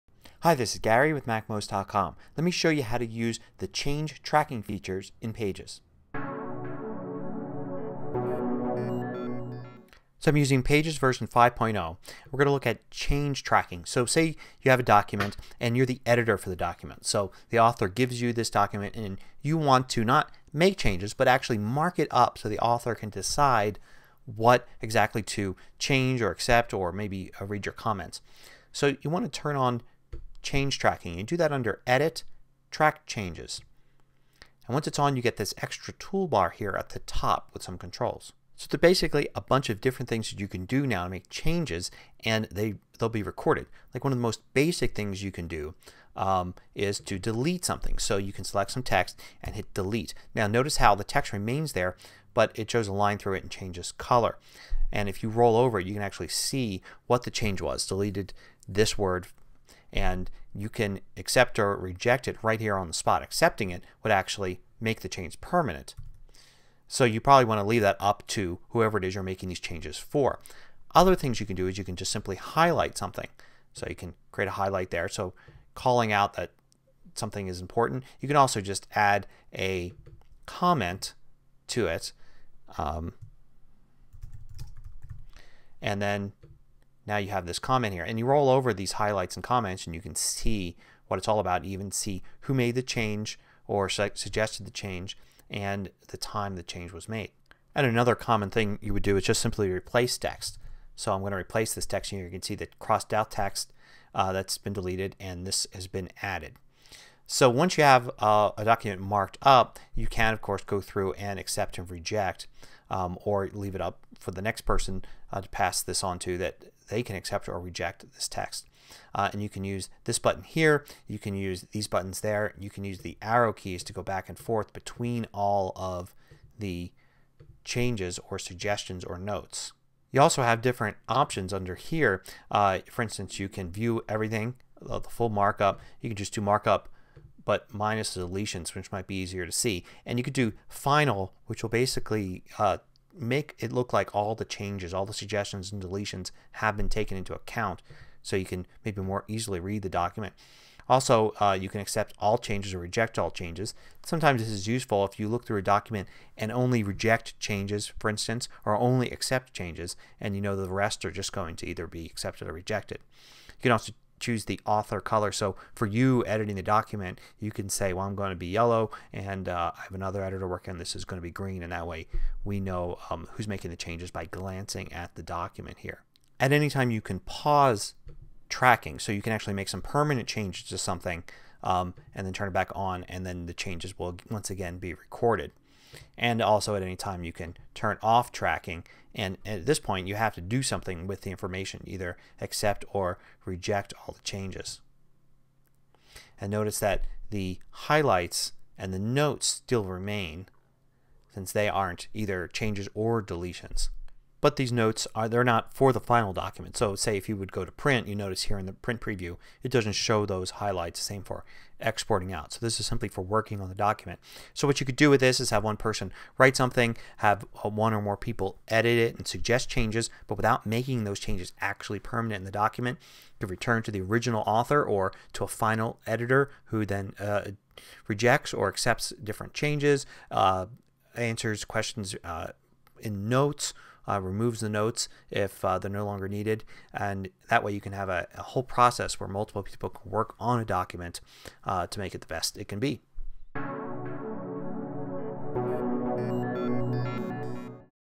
Tracking Changes In Pages Documents (#983)
You can use the change tracking feature in pages to pass a document between multiple writers or editors and see suggestions from each. Change tracking will record deletions, changes and notes with the name of the editor. You can review the changes and incorporate them into the final document.